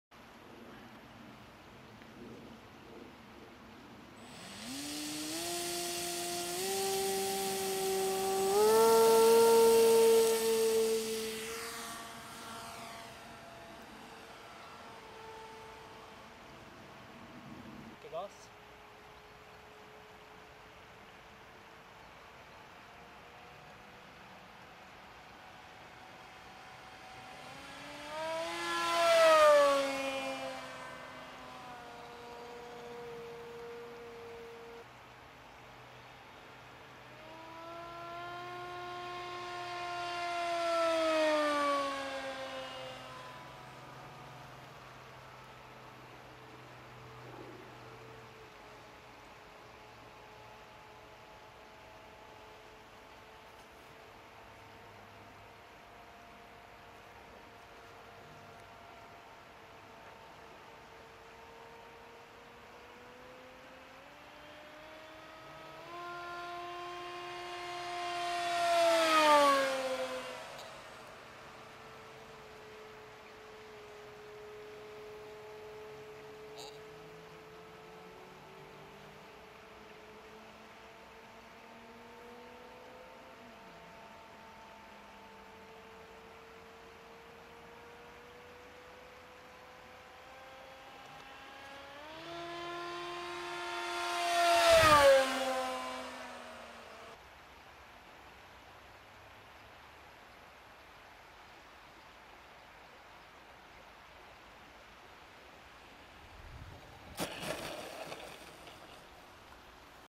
Fast rc plane
Finally the LR-1 Racing Devil is working like it should. The speed is about 150kp/h. Soon I will increase the power from 700 watts, to almoust 2000 watts. Thanks to my brother for filming.